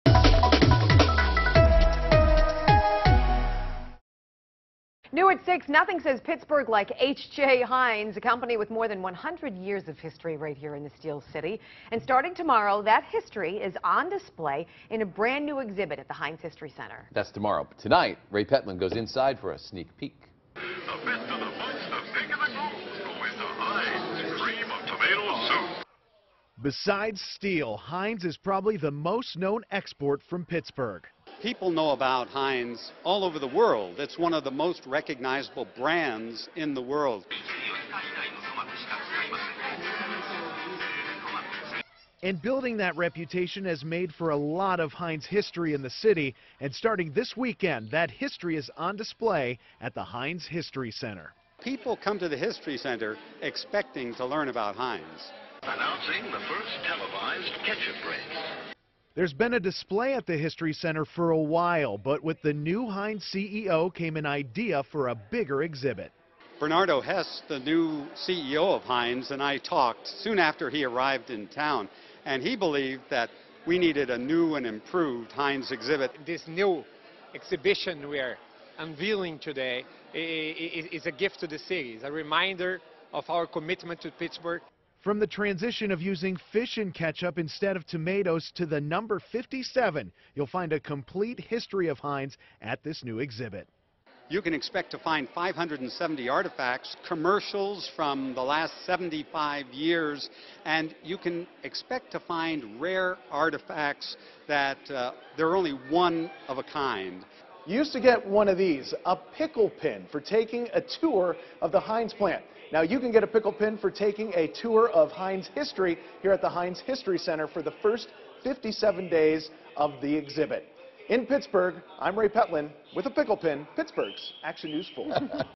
Heinz History Center unveils new Heinz exhibit
Heinz products are among the most know exports from Pittsburgh, and the Heinz Company has a long-standing history in the city.